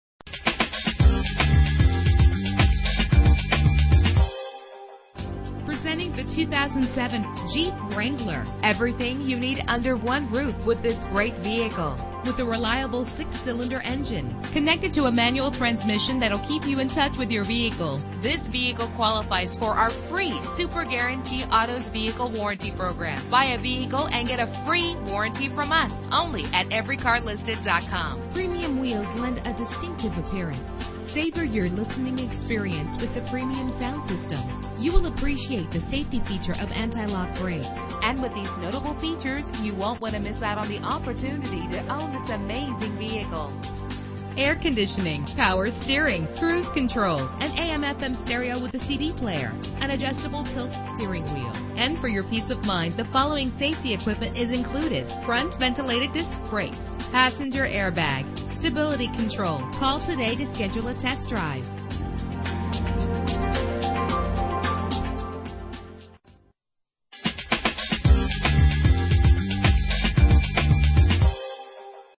2007 Jeep Wrangler - ROSWELL GA
Year: 2007
Make: Jeep
Model: Wrangler
Trim: Sahara
Engine: 3.8 liter 6 cylinder 12 valve
Transmission: 6-Speed Manual
Color: Burgundy
Mileage: 38271
Address:
11185 ALPHARETTA HWY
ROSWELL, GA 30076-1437